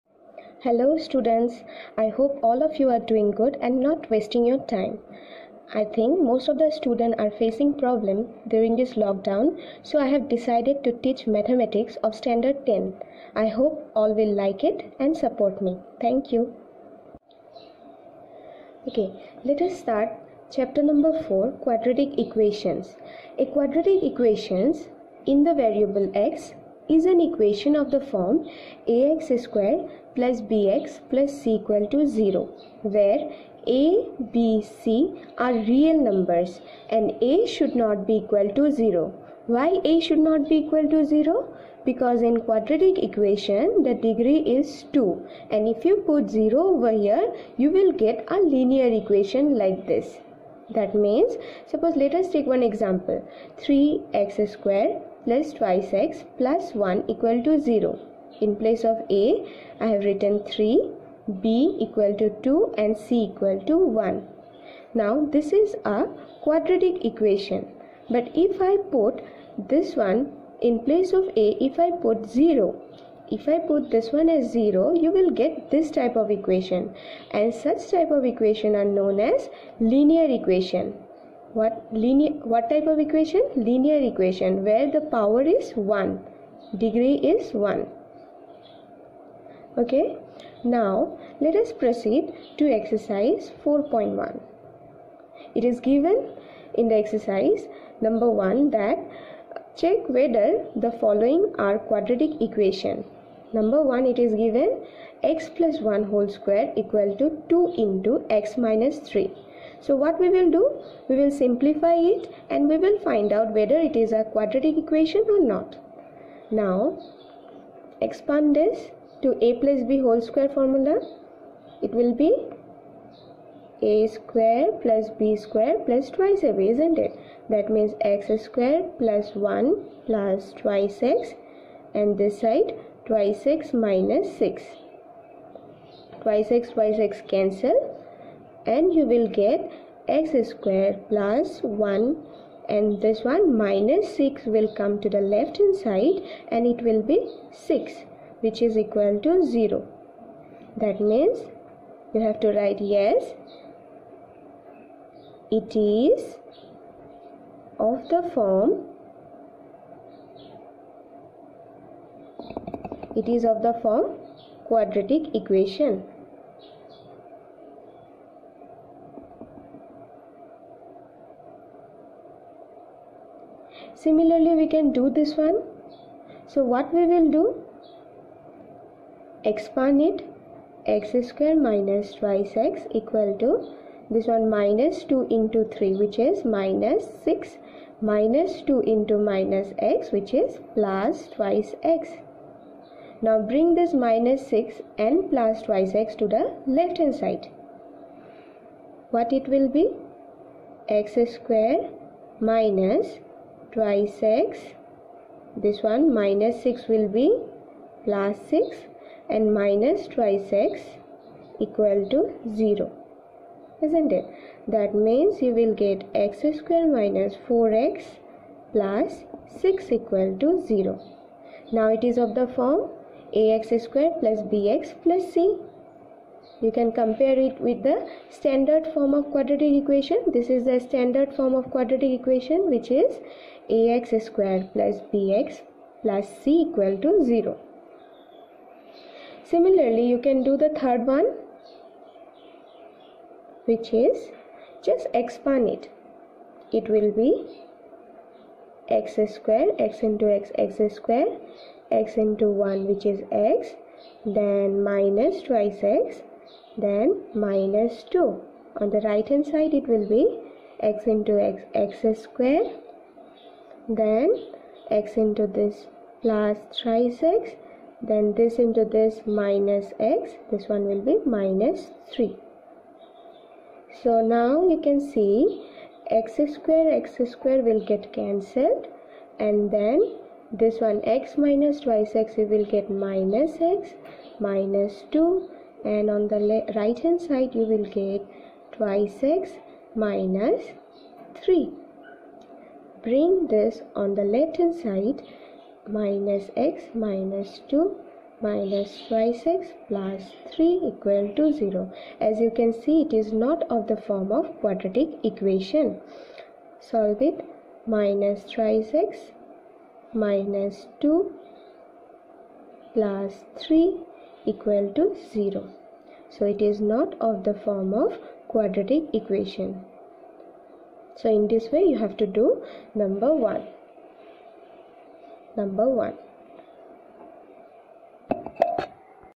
Ex-4.1Question no.1 i to iii|Quadratic Equations|CLASS 10 Mathematics
Class 10 Mathematics Ncert Chapterwise disvussions ||Let's Solve